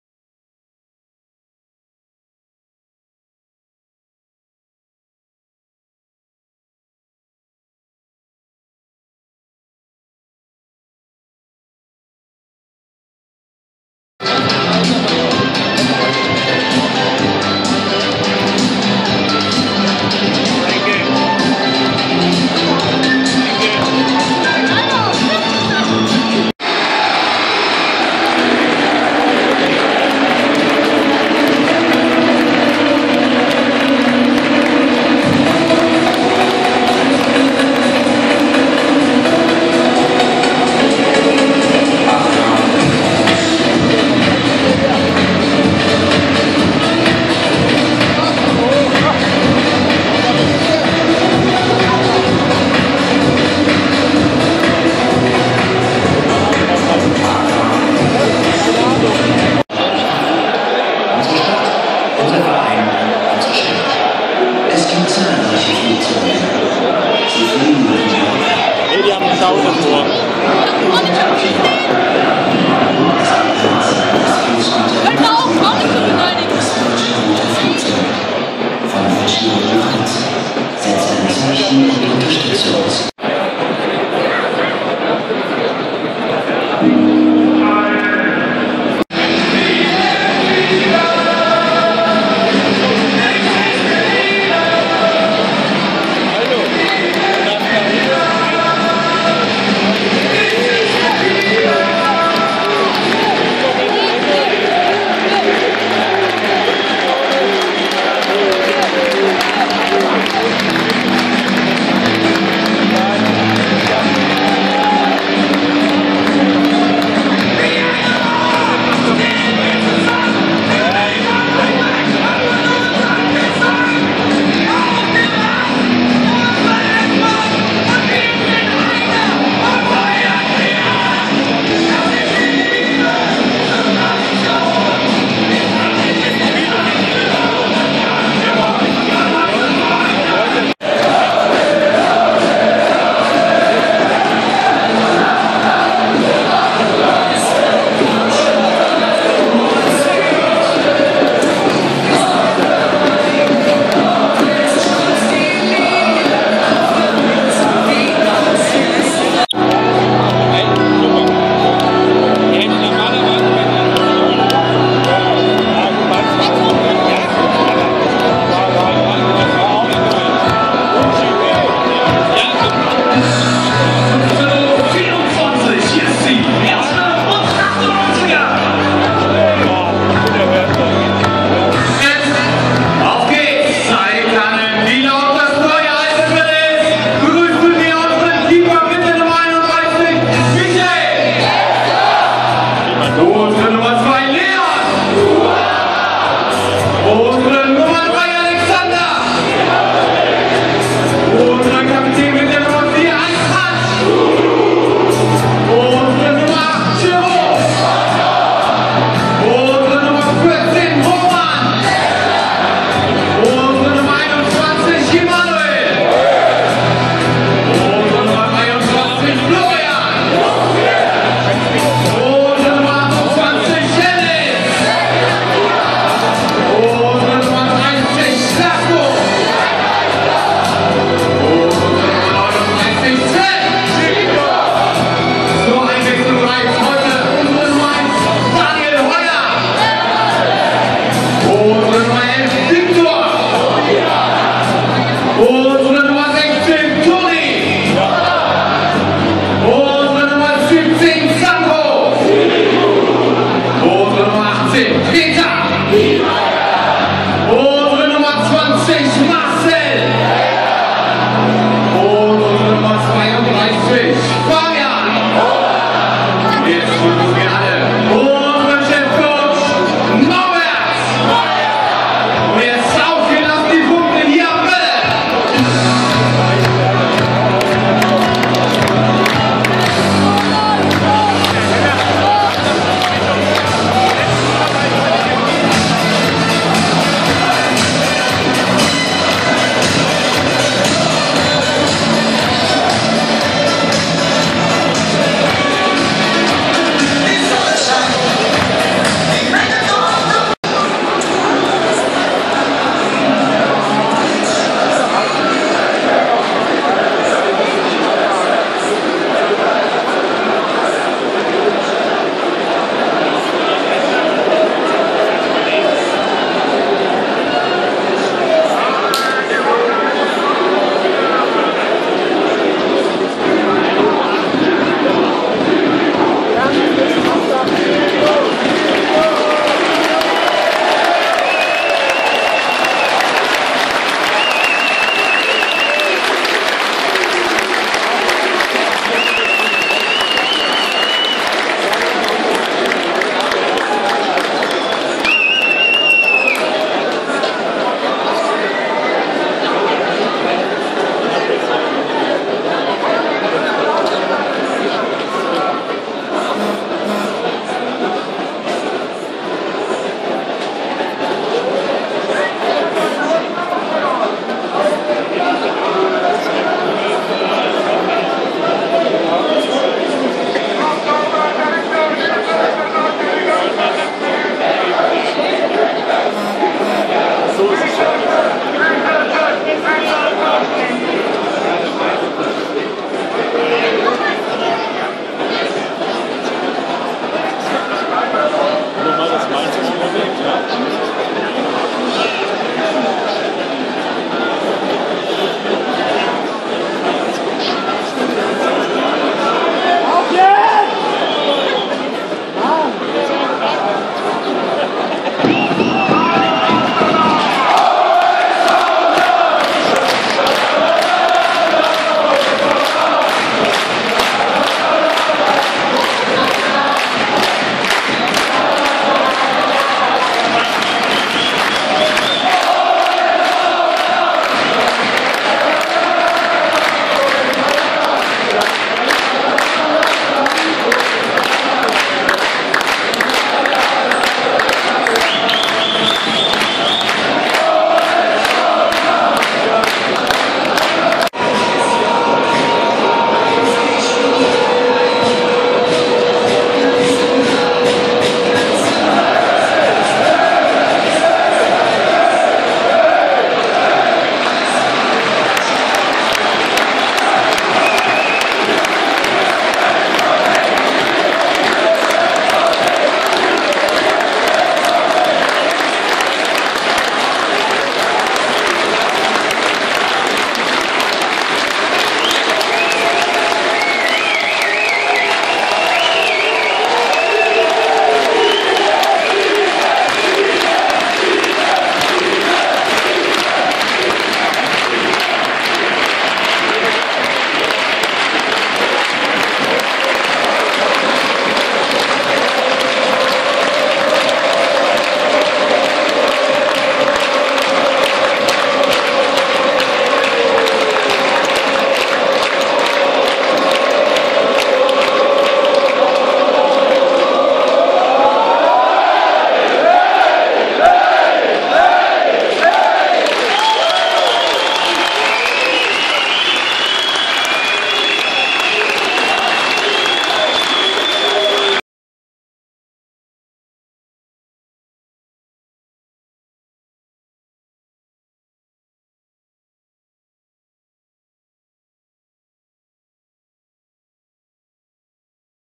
SV Darmstadt 98 - VfL Wolfsburg
Dritter Versuch, ein Video von dieser Partie zu posten

Es sind KEINE Ausschnitte aus dem Spiel zu sehen, lediglich die Stimmung der Fans VOR und NACH dem Spiel und das Warmmachen der Mannschaften.